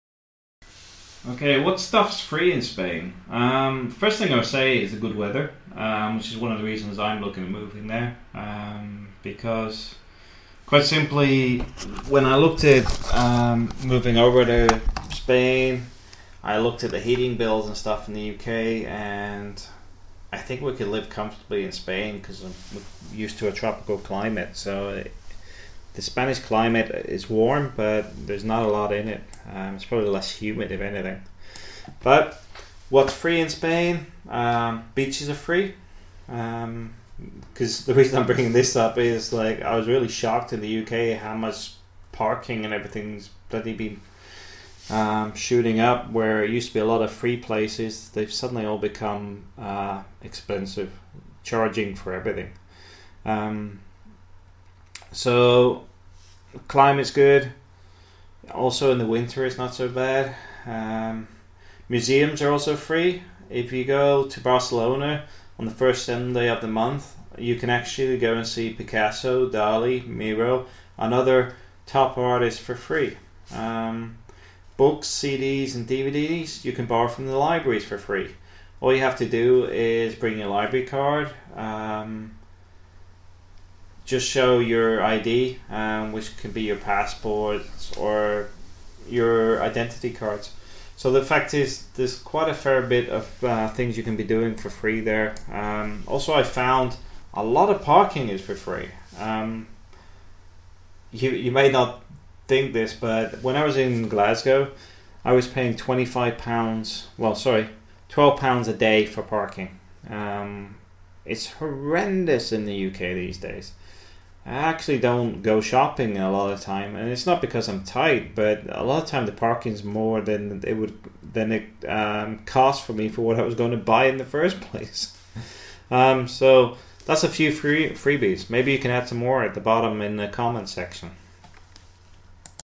Top tips for moving to Spain - What can you get for free in Spain
If your visiting or looking to move to Spain what sort of things can you expect for free?